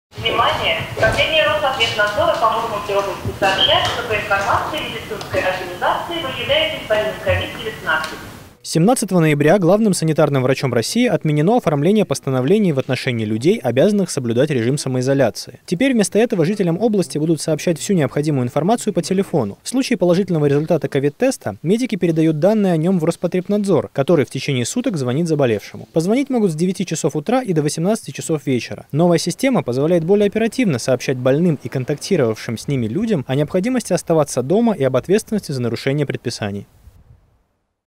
В Мурманской области представлена система автоинформирования о результатах тестов на COVID-19
Программа будет уведомлять жителей Заполярья в случае положительного результата на коронавирус.